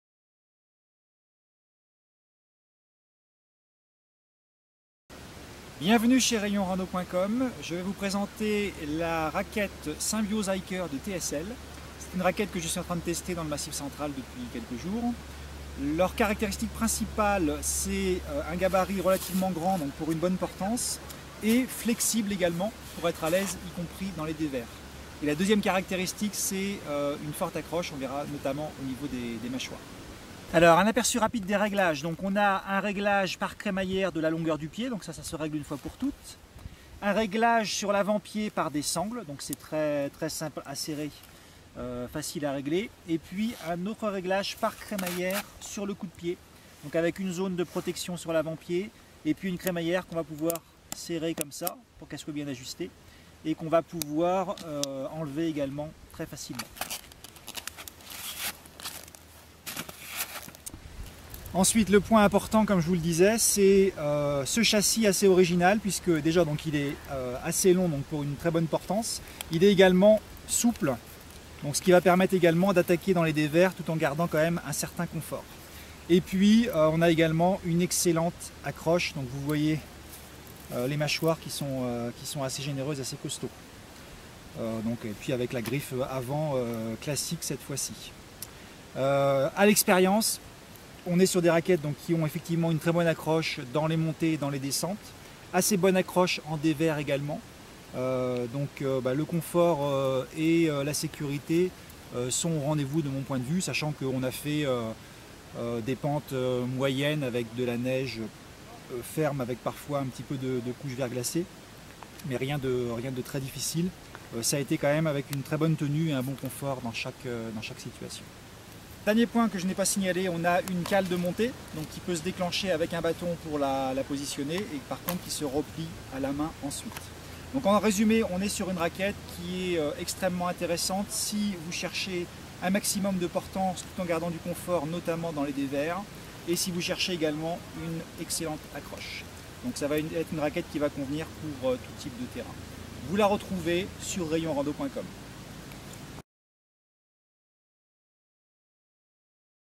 Rayonrando.com : Présentation des raquettes à neige Symbioz Hiker de TSL
Raquettes à neige flexibles s'adaptant aux aspérités du terrain et permettant un déroulé du pied naturel. Très forte accroche même sur les terrains difficiles.